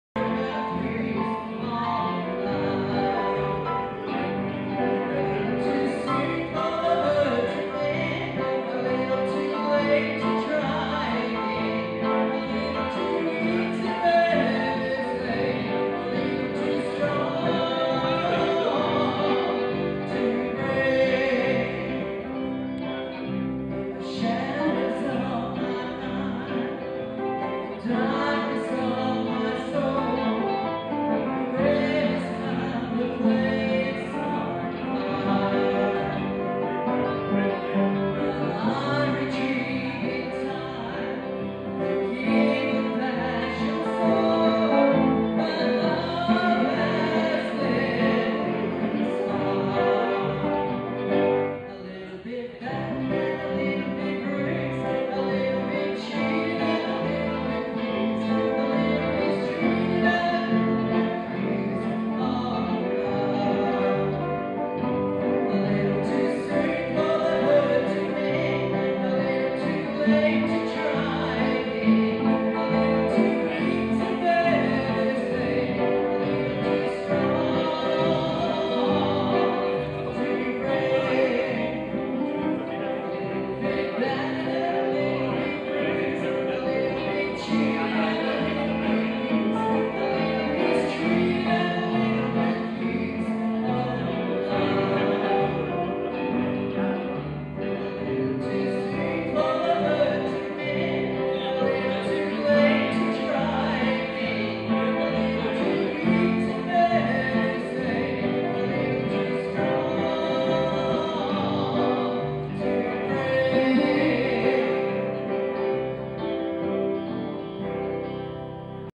au 4 moe music festival 2011 gippsland country music
held in heritage town great place to visit normally but magic with the music worth a long drive to catch this festival